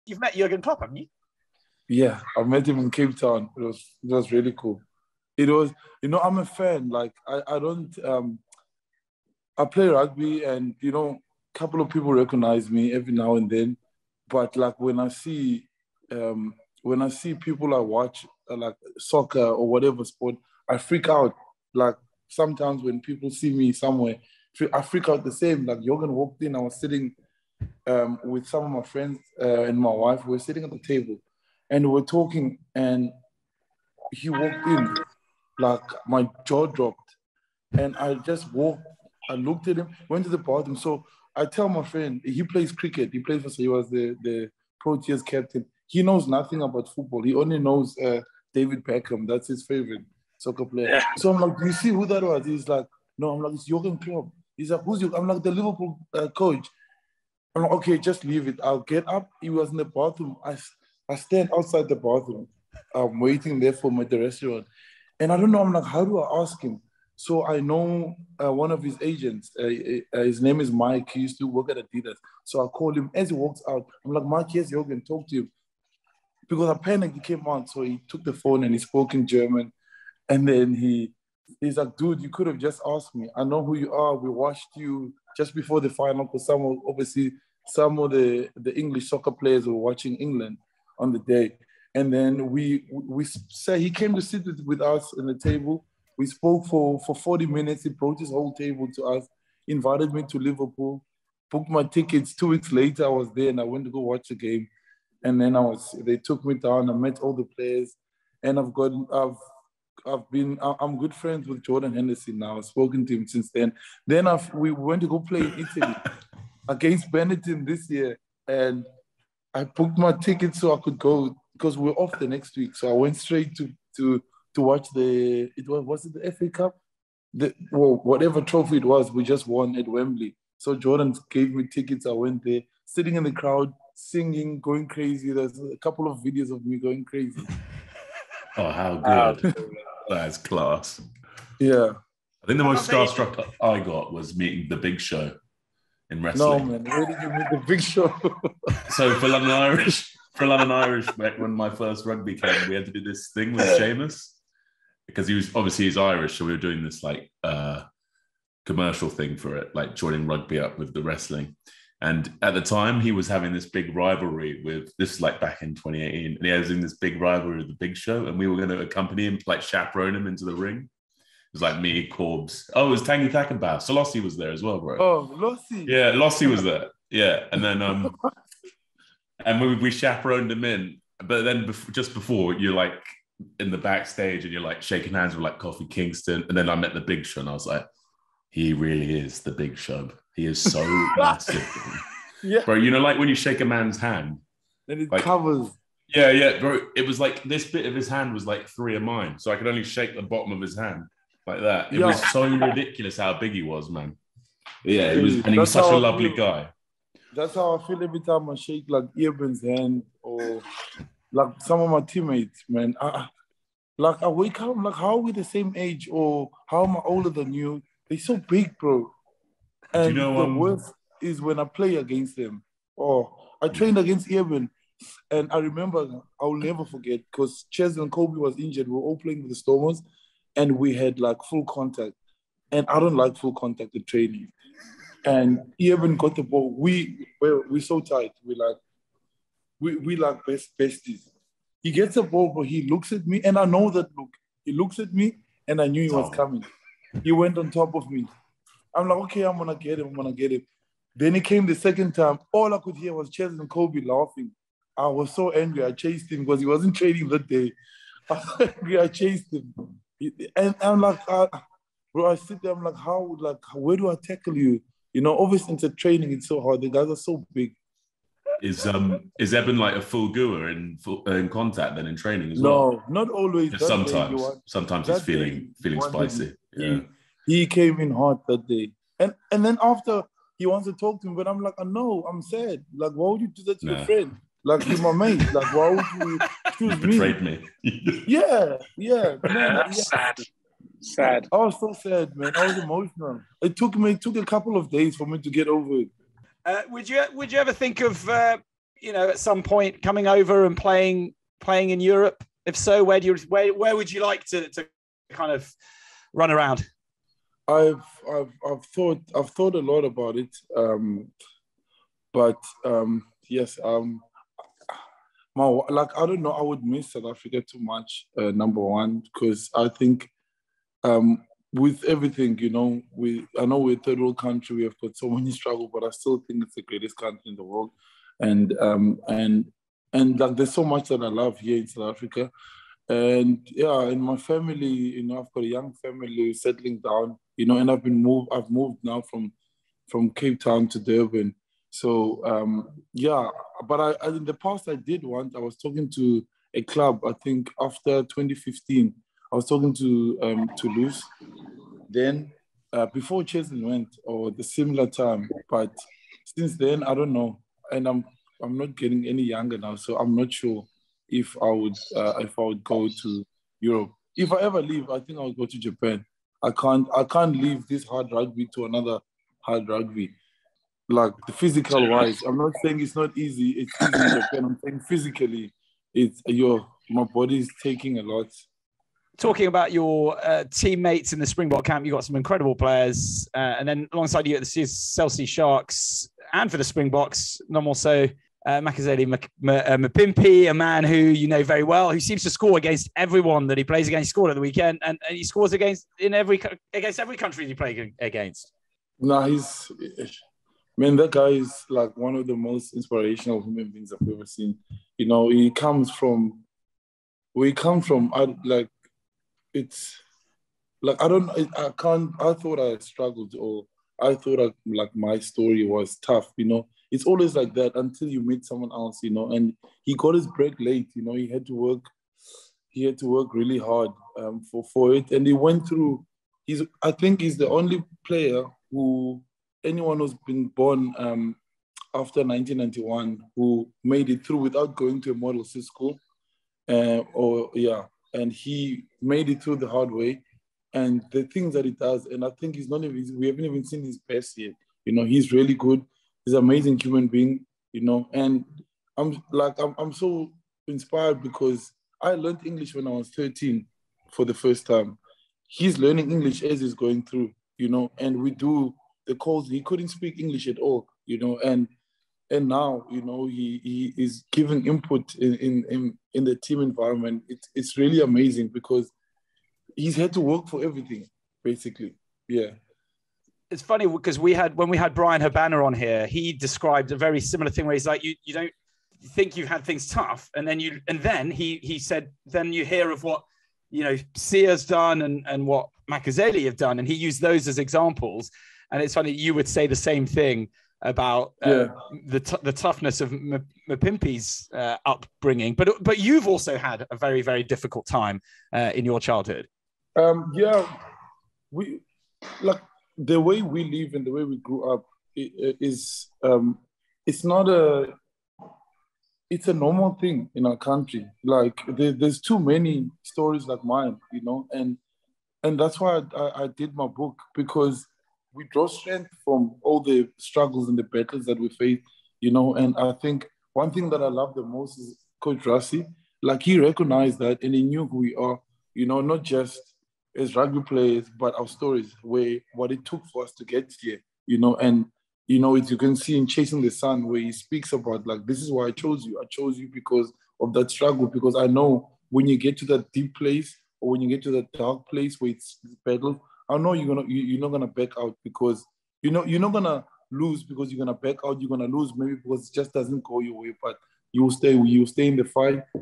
Siya Kolisi sums up training with Eben Etzebeth in brilliant story | RugbyPass Offload
Springbok rugby world cup champion Siya Kolisi spoke to RugbyPass Offload this week on the podcast. He spoke about his experiences with Roc Nation, meeting Liverpool manager Jurgen Klopp. He also spoke about what it was like training with man-mountain Eben Etzebeth.